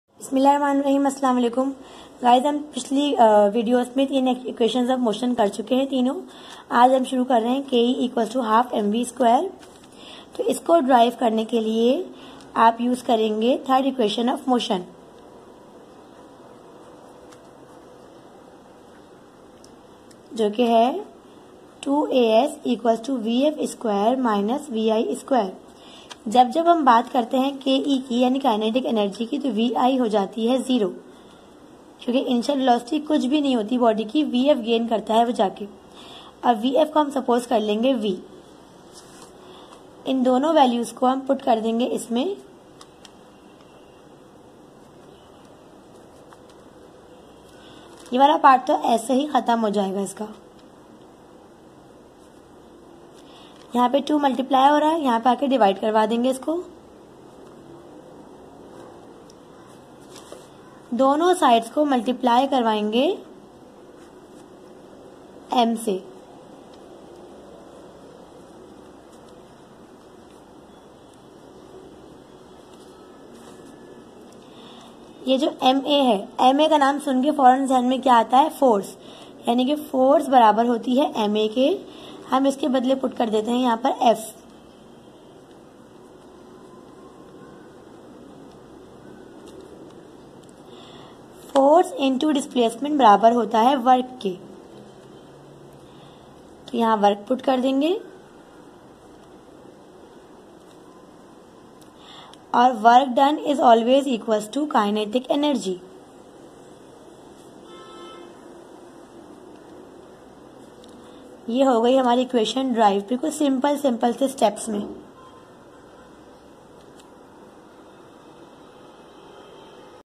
Derivation for KINETIC ENERGY of a body in simple & easy steps
K.E=1/2mv2...physics class 10th...(p.s)a little theory/wordings must be added in equation.